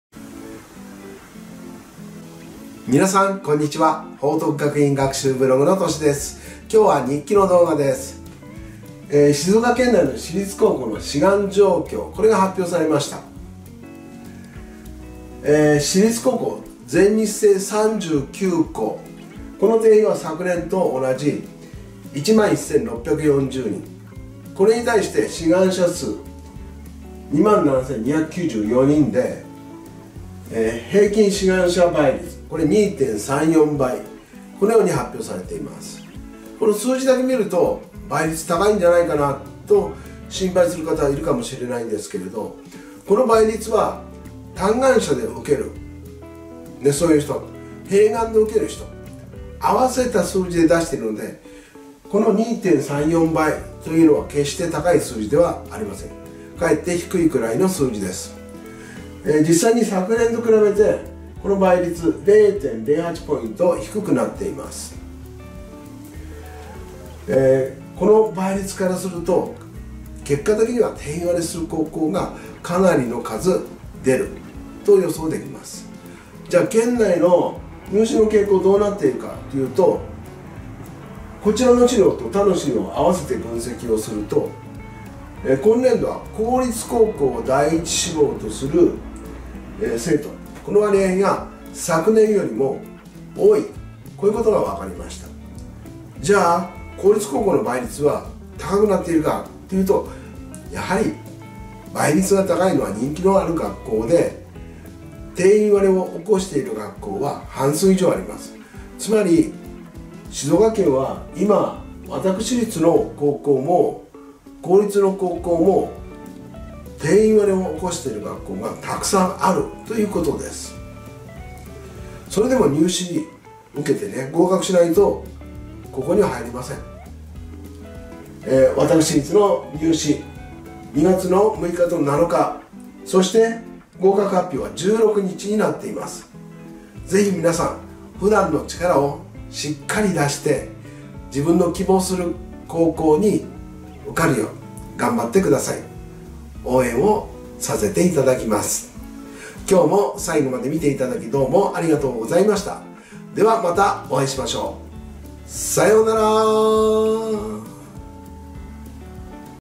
静岡県私立高校志願状況発表
静岡県私立高校志願状況発表が発表されました。その内容と、昨年と比較してどうなのか。今回の高校入試の傾向についてお話ししました。